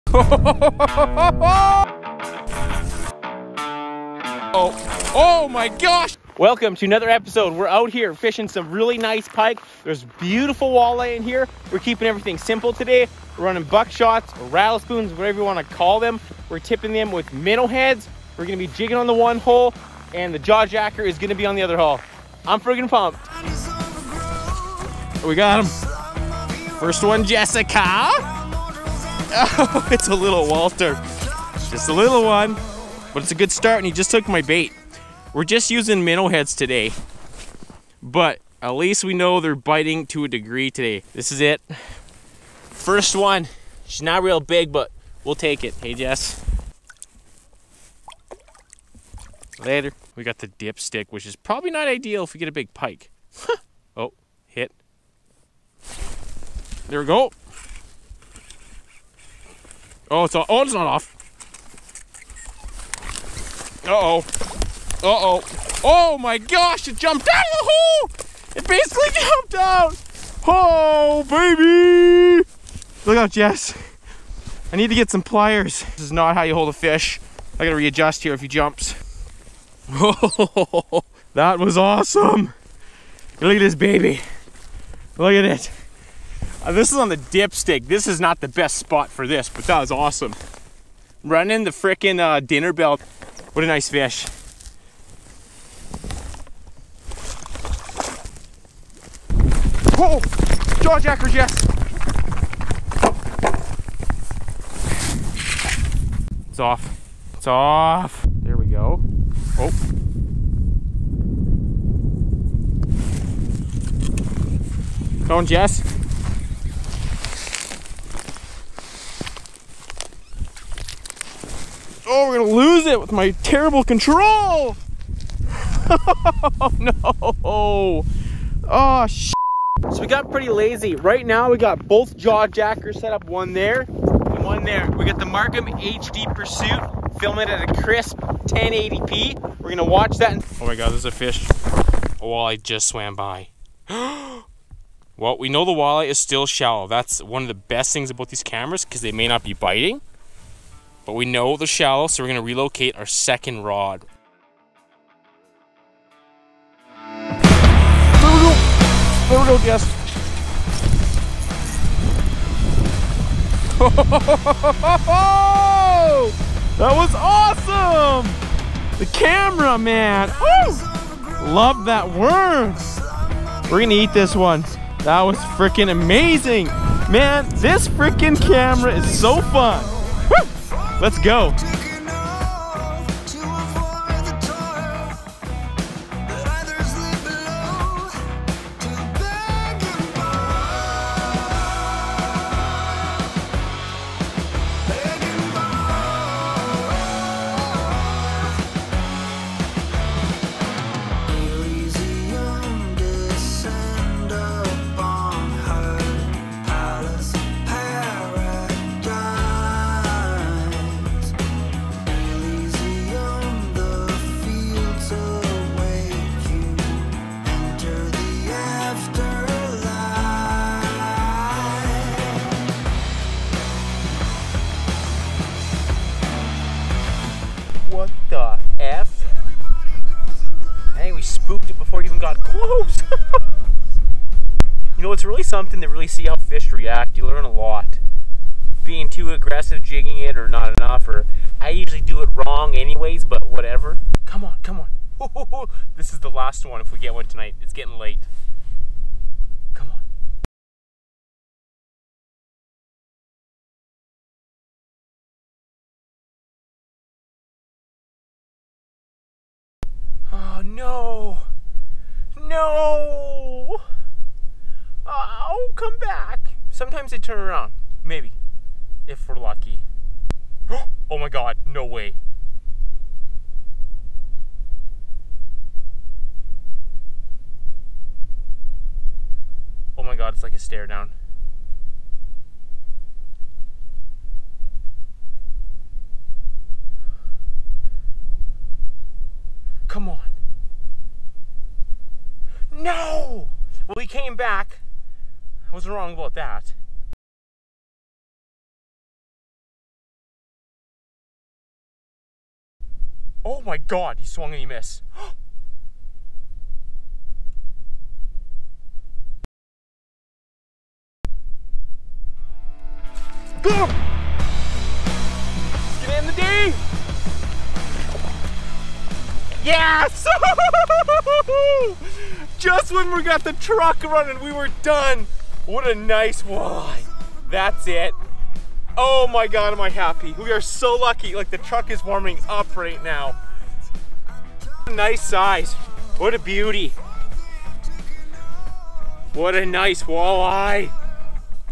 Antelope Lake Big Walleye AND Northern Pike Crystal Clear Underwater Footage MarCum HD Pursuit
walleye northernpike marcumHD Fishing Antelope Lake with Jaw Jackers and the crystal clear Marcum HD Pursuit! Lots of big Walleye's and Northern Pike! Looking for a new PB! We use Frostbite gear and minnows for bait. Still on early ice with lots of action!